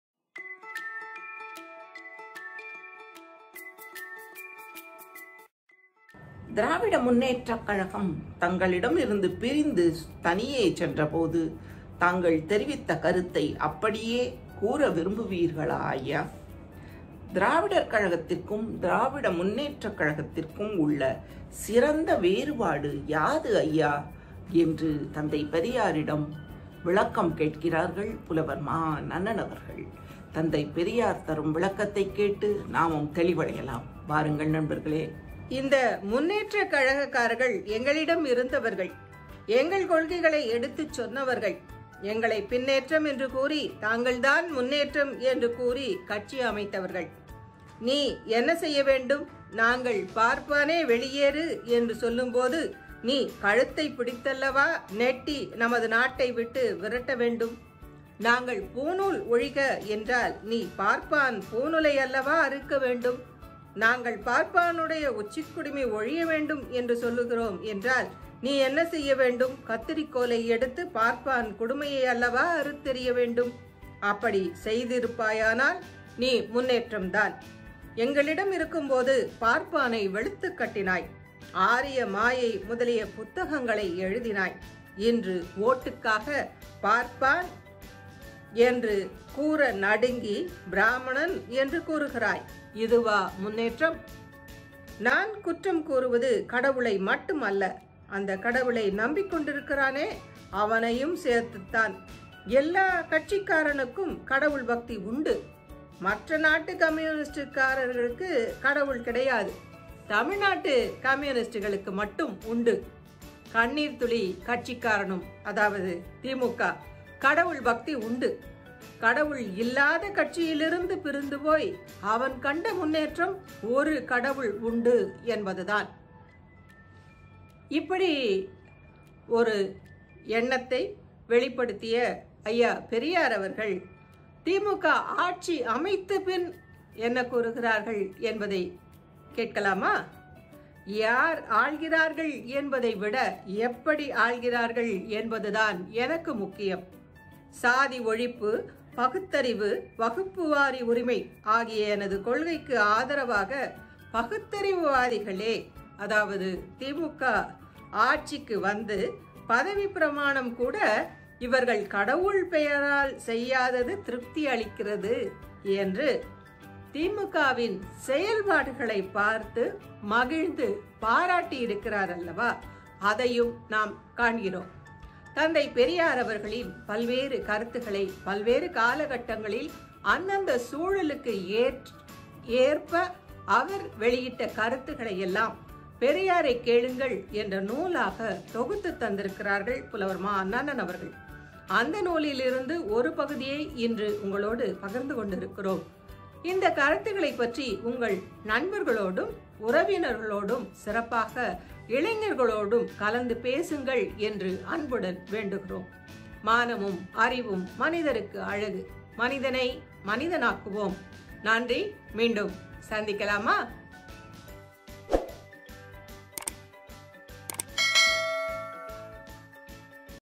திராவிடர் கழகம் , திராவிட முன்னேற்றக் கழகம் இரண்டுக்கும் உள்ள வேறுபாடு பற்றி தந்தை பெரியார்
பெரியாரைக் கேளுங்கள் 668/புலவர் மா நன்னன்